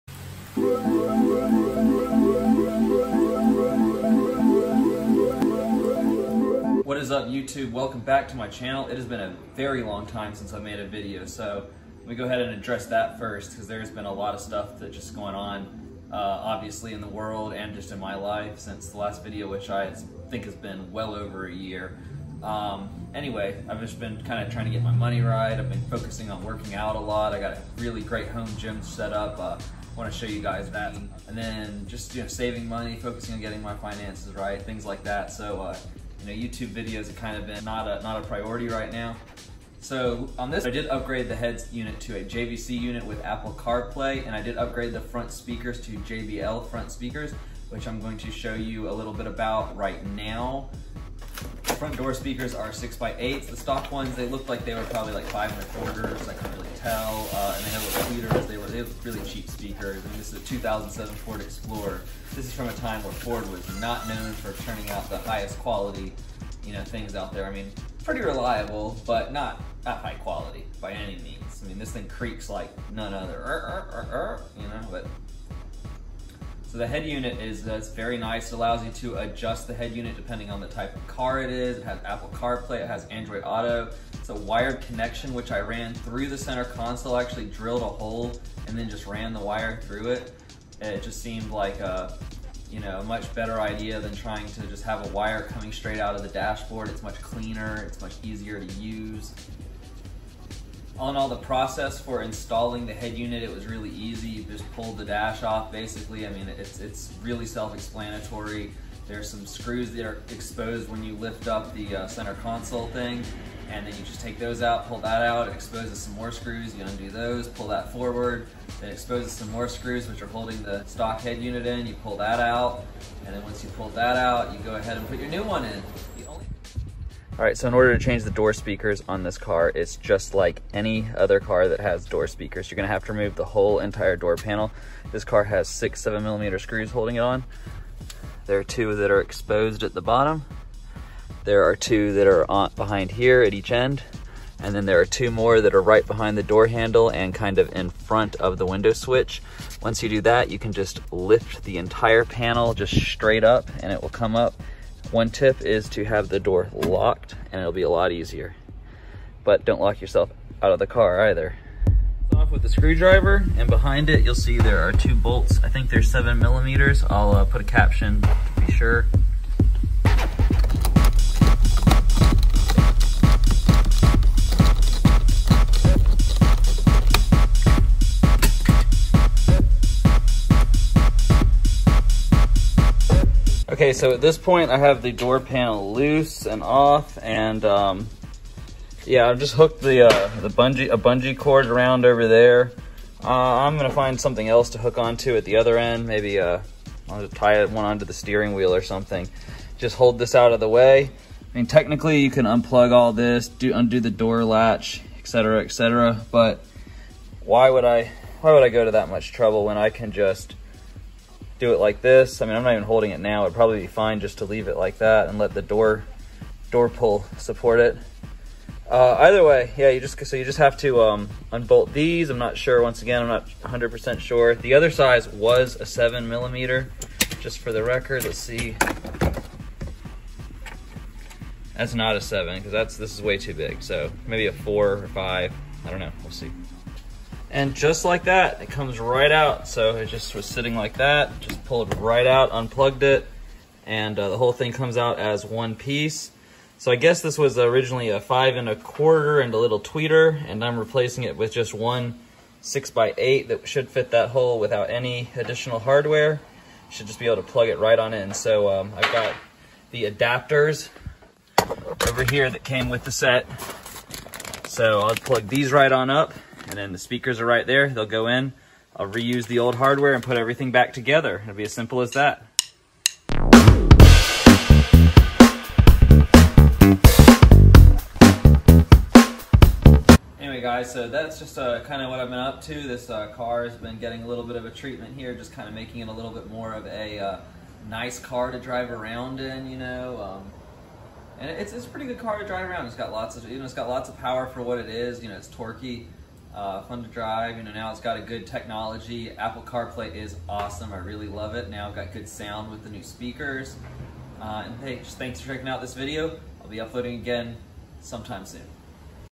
Upgrading to Apple Car Play and New Door Speakers! (Ford Explorer)
In this video I review the installation and upgrade of my JVC M560BT headunit and the upgrade and installation of my JBL 8621F 6X8 door speakers for my 2007 Ford Explorer daily driver. This upgraded head unit includes Apple Car play and Android Auto which is an excellent way to bring an older car into the modern era. Apple Car Play and Android Auto make the head unit of the car essentially your phone for all essential functions such as calling, gps, music, etc. The upgraded speakers make an excellent improvement in overall sound quality and are excellent speakers for the money.

What you will need:
Head Unit
Trim Tools